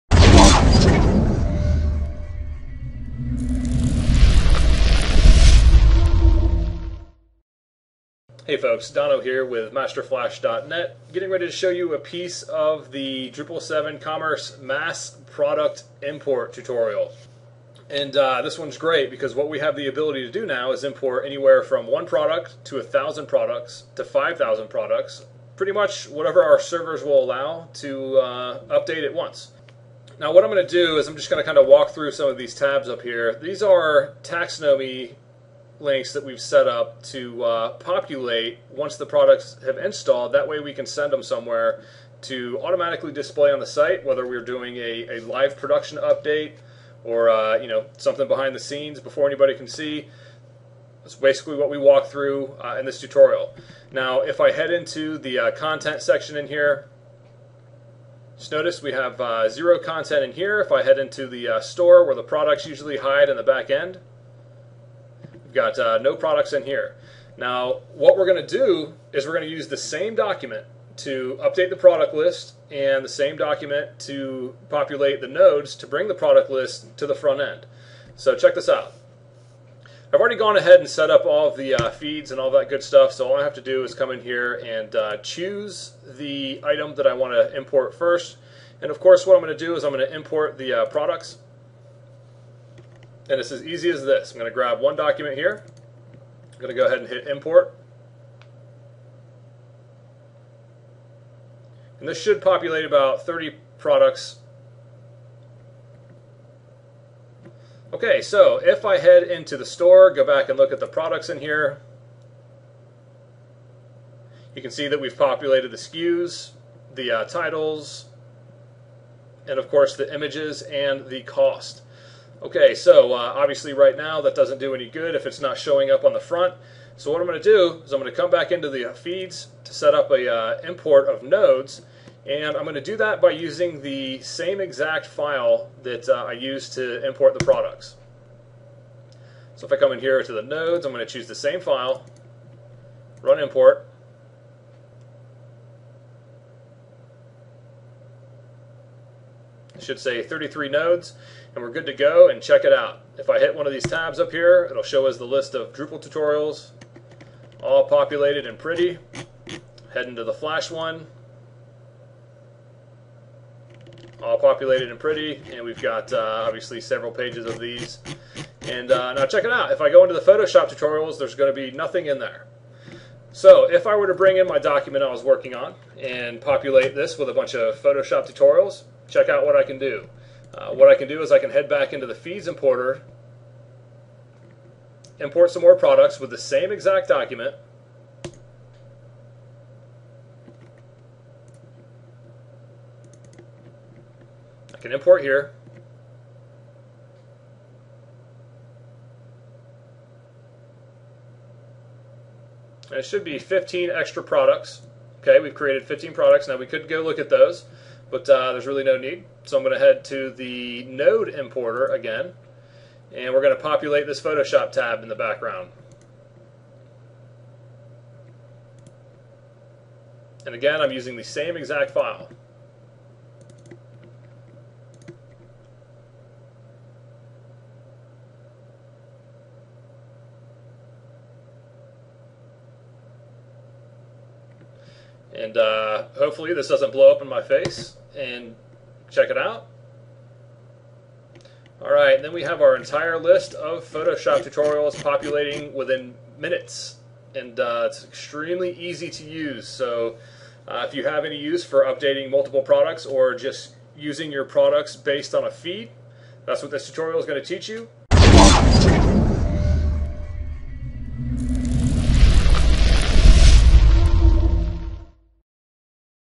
Drupal 7 Commerce Product Import Tutorial using Commerce Feeds
DEMO VIDEO ONLY - Not the full tutorial

Learn how to populate products in Drupal 7 Commerce making use of Commerce Feed and the Feed Module. Learn how to set up your importers from scratch on Custom Product Types as well as Content Types to populate the front end of your store. You will also learn how to populate your taxonomy based on the products you import all at the same time. We cover a smooth flow of importing and setting up of products through .csv and .txt files. This allows you to update anywhere from 1 product - 3000 products and even more all at once.